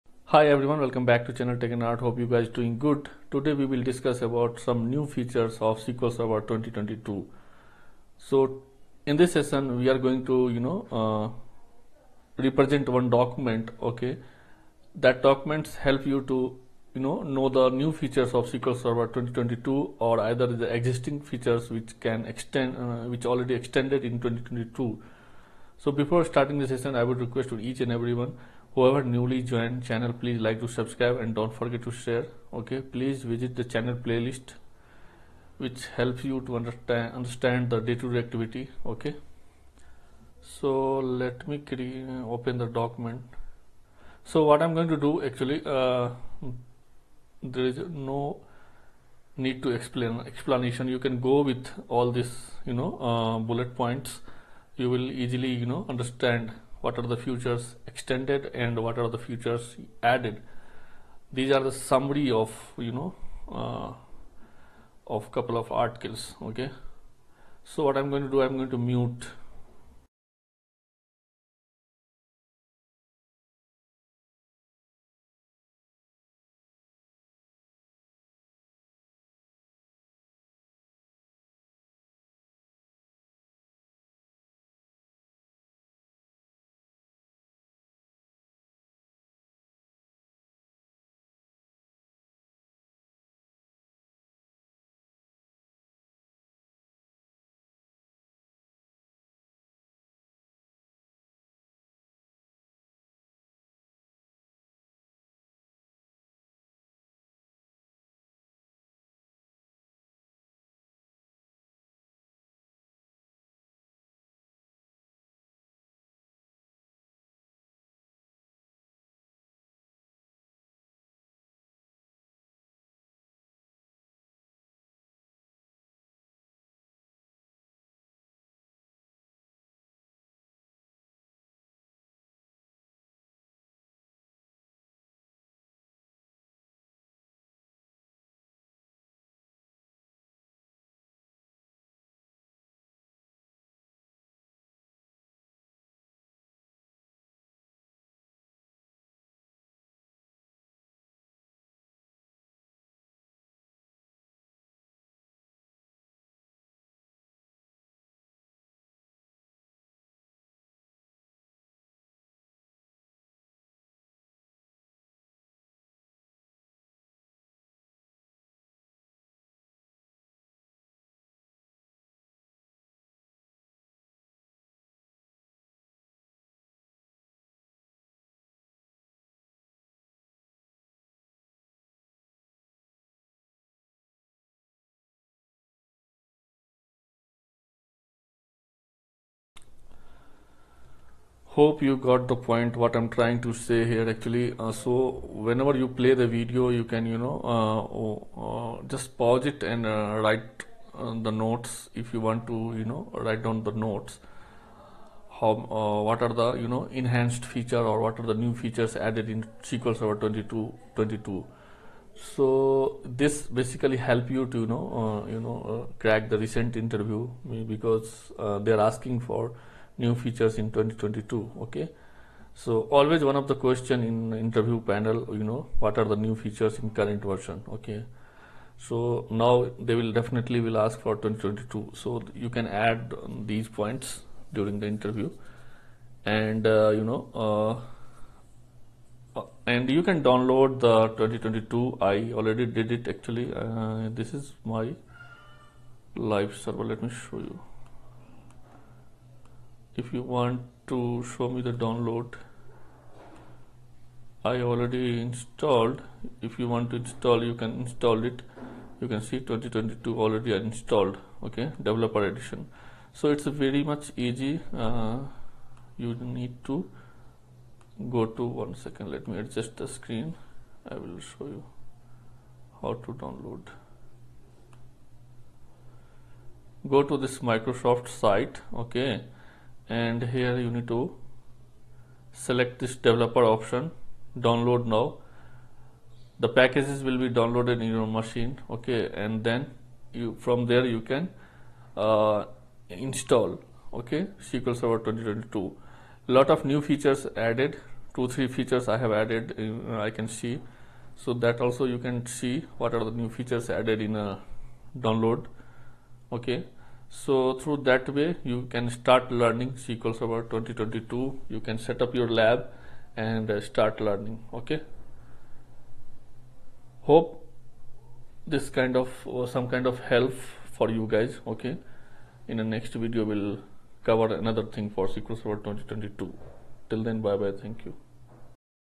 SQL Server 2022 New Features || SQL Server 2022 Download || SQL Server 2022 add on
In this session we are going to discuss about SQL Server 2022 New Features | SQL Server 2022 || SQL Server 2022 Download || SQL Server 2022 add on

SQL MIRRORING

SQL REPLICATION